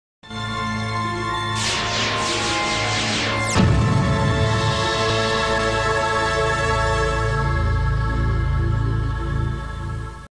Gloryhoundz Logo (Redone)
The logo is an opposite side of Mizfit/Ohana channel, my sincere apologies to Mizfit/Ohana fans, I know I don't like it when Mushu, Jim Hawkins, Hercules and Aladdin steals Ariel from Eric but I respect your opinion.